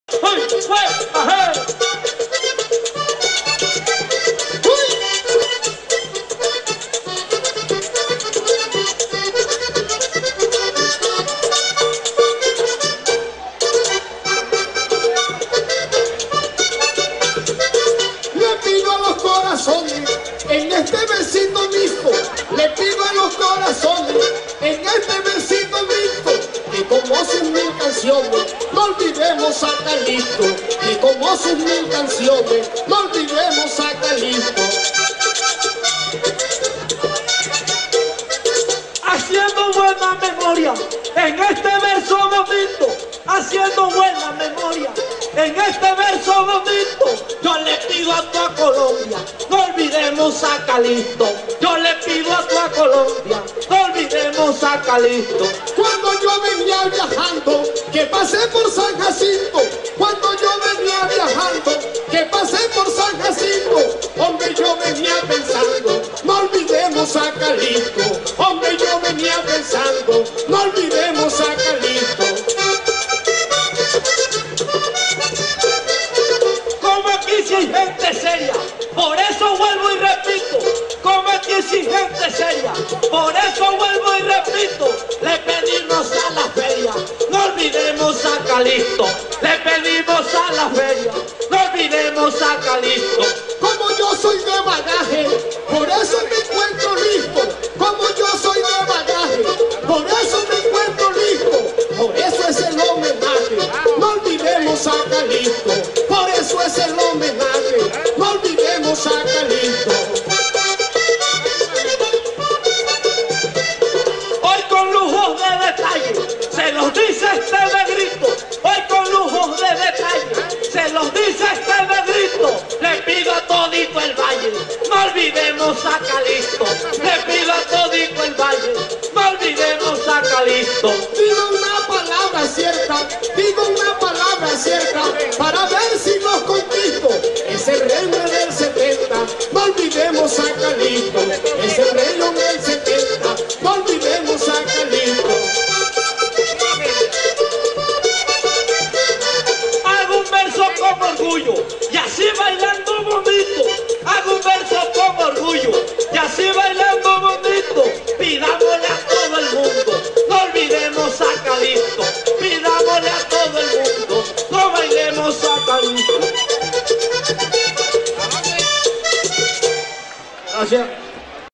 HOMENAJEANDO A CALIXTO OCHOA, PIQUERIA VALLENATA EN VALLEDUPAR
En este video se puede apreciar el repentismo de dos grandes verseadores, en modalidad pie forzado con el tema "No olvidemos a Calixto", en el marco del festival de la leyenda vallenata en Valledupar Cesar.